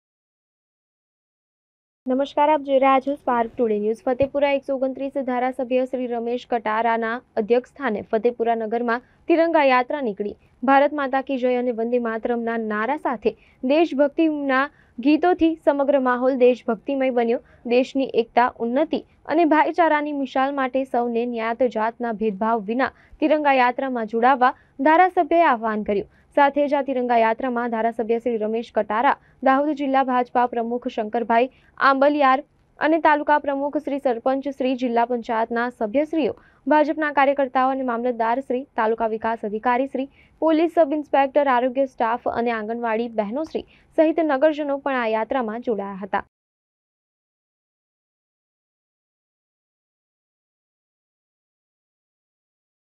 ફતેપુરા 129 ધારાસભ્ય શ્રી રમેશ કટારા ના અધ્યક્ષસ્થાને ફતેપુરા નગર માં તિરંગા યાત્રા નીકળી 2024
ફતેપુરા 129 ધારાસભ્ય શ્રી રમેશ કટારા ના અધ્યક્ષસ્થાને ફતેપુરા નગર માં તિરંગા યાત્રા નીકળી